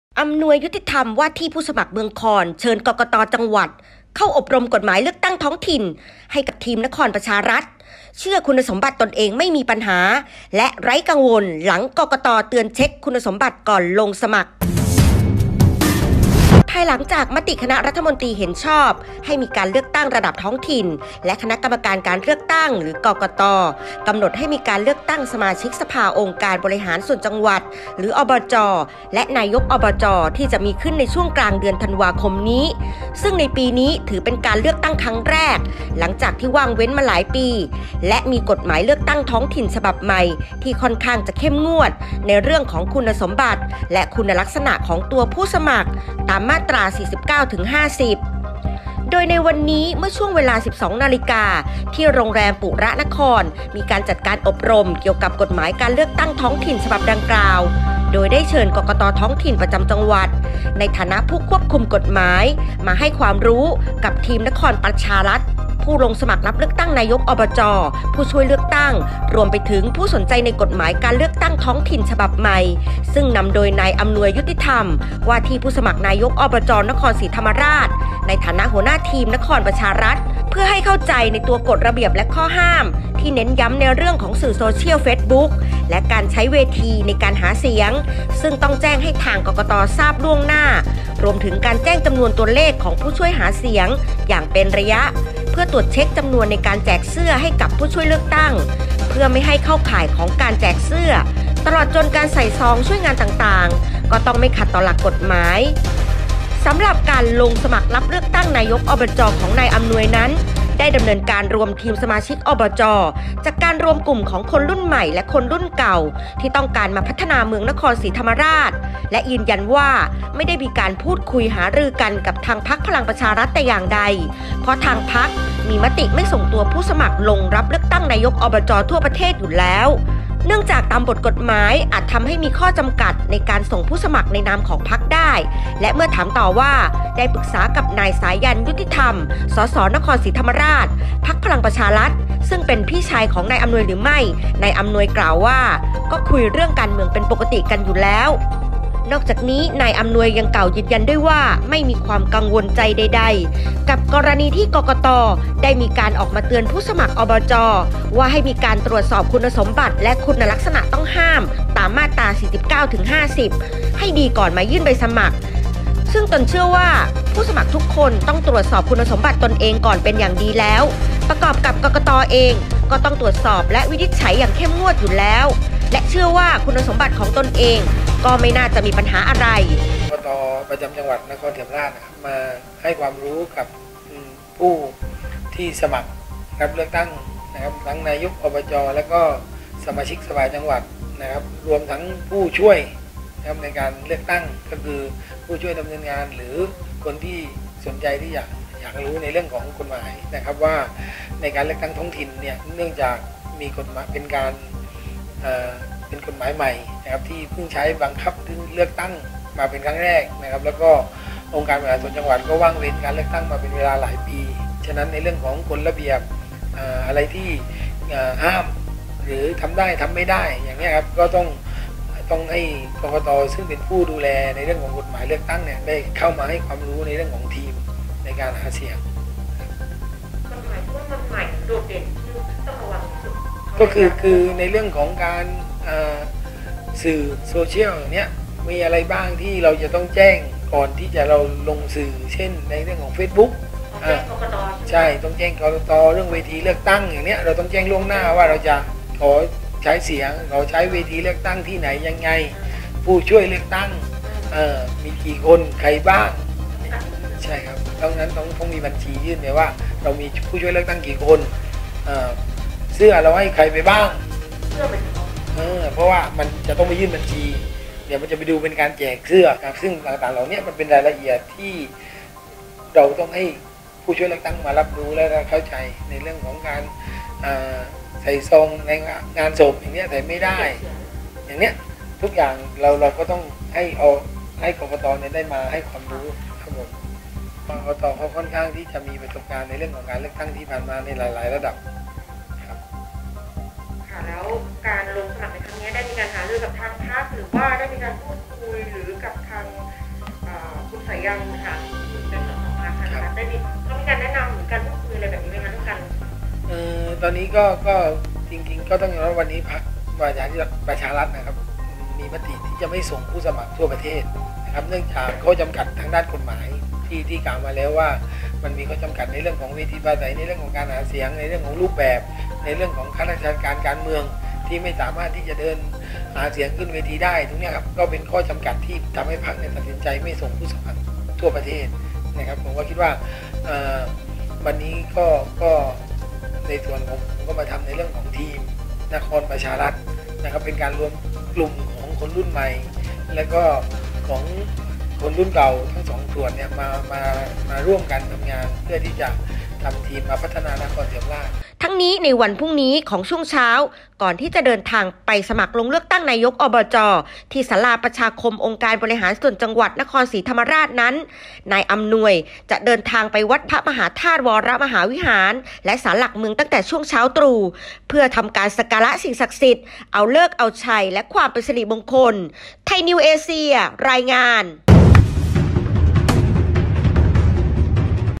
อำนวย ยุติธรรม
“อำนวย ยุติธรรม” ว่าที่ผู้สมัคร เมืองคอน เชิญ กกต.จังหวัด เข้าอบรมกฏหมายเลือกตั้งท้องถิ่นกับทีมนครประชารัฐ